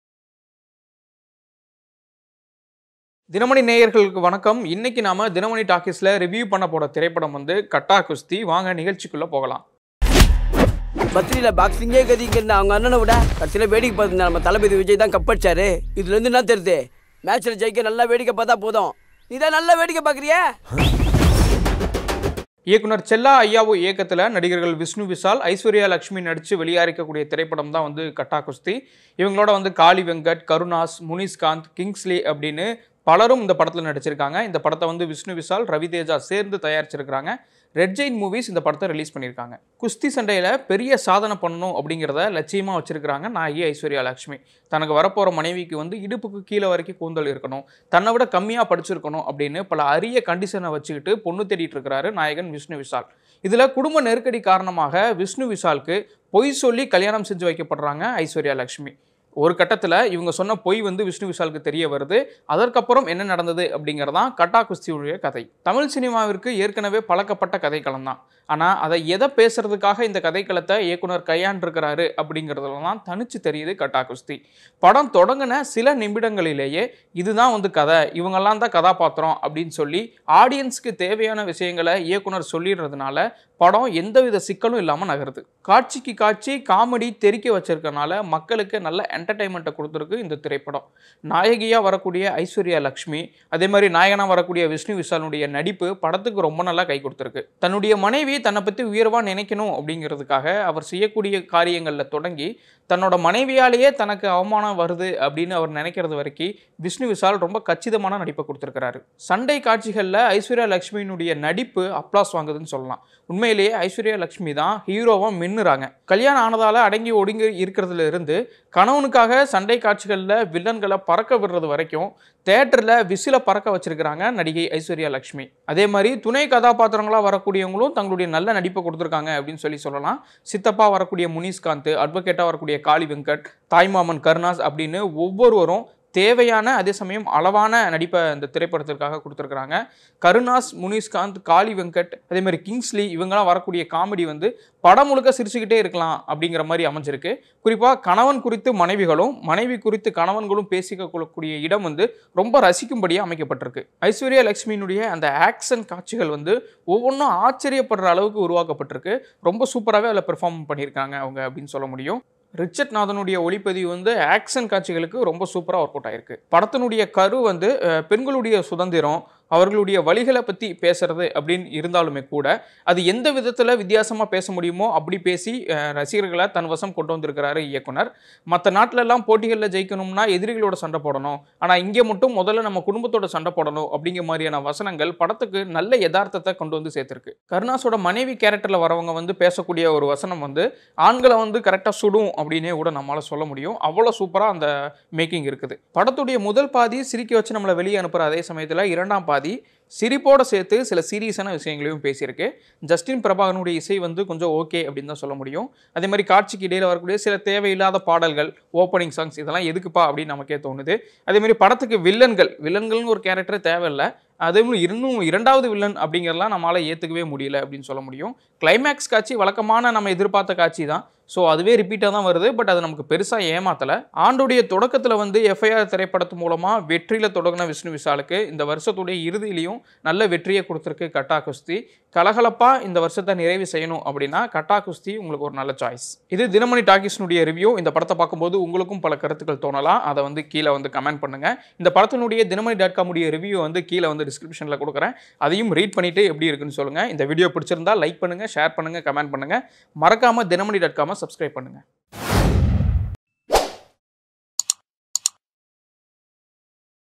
ரசிகர்களைக் கவர்ந்ததா கட்டா குஸ்தி? Gatta Kusthi Movie Review | Dinamani
Dinamani Daily Seithigal Morning:
Dinamani Daily Seithigal Evening:
Dinamani Daily Breaking News:
Hourly Headlines: